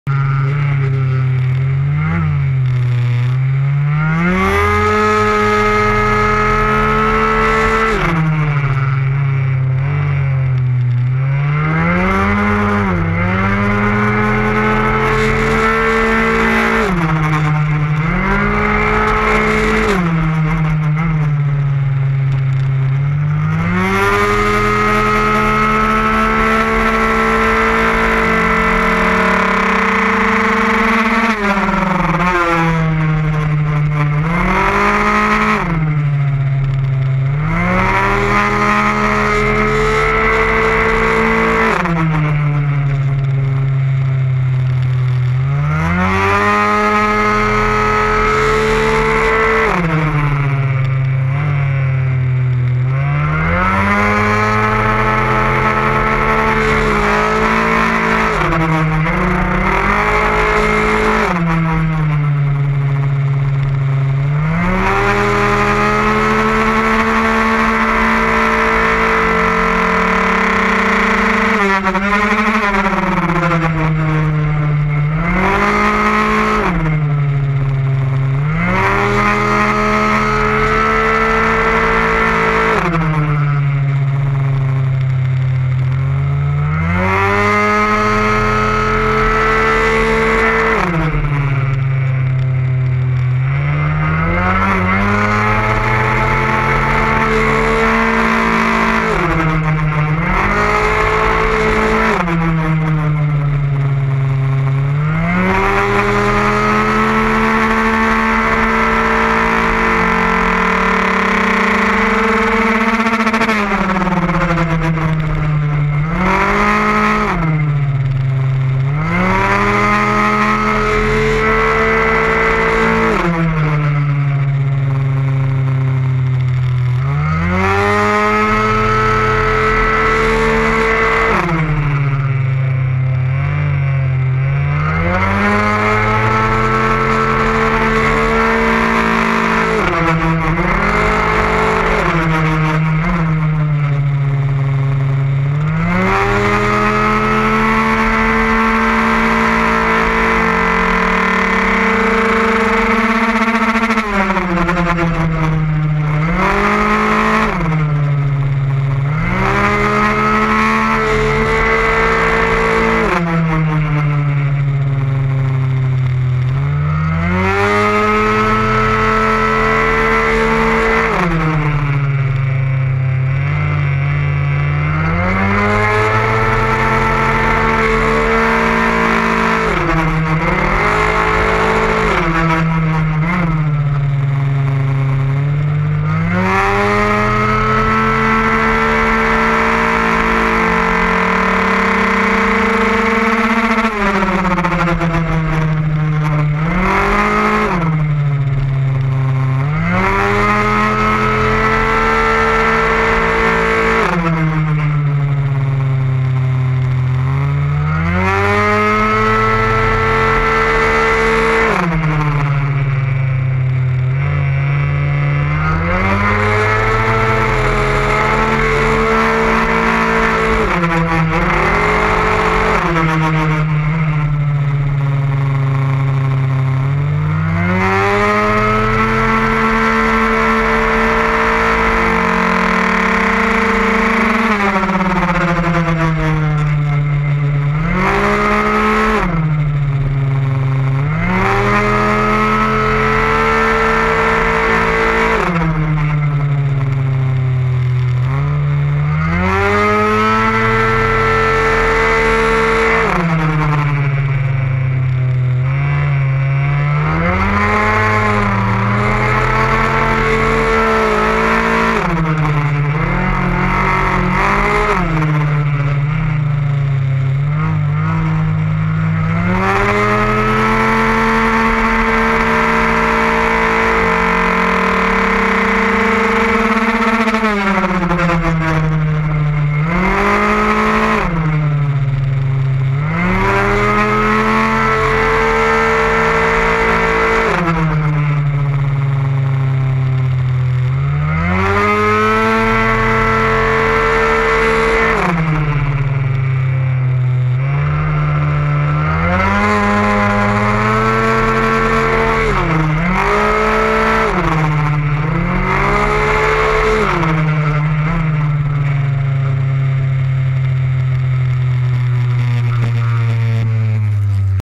Essai Libre#1 Piste de Corcoué sur Logne Samedi 12 Avril 2014
Pilote Julien Mach G Proto Scoot 50cc Malossi Team Cup